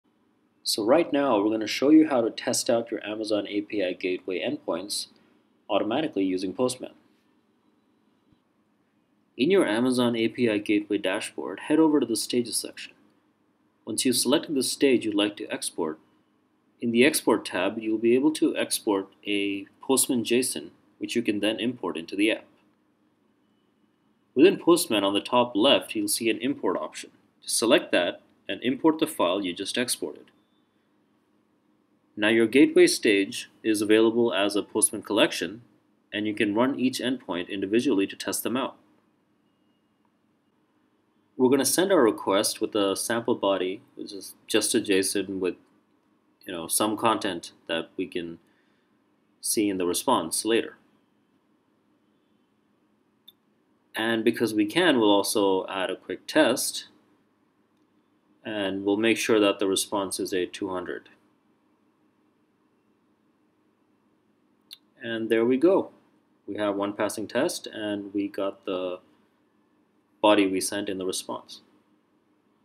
Run & test your AWS API Gateway endpoints with Postman
You can export your AWS API Gateway stages as a Postman Collection. After importing, Postman allows you to add scripts & tests and whole lot more!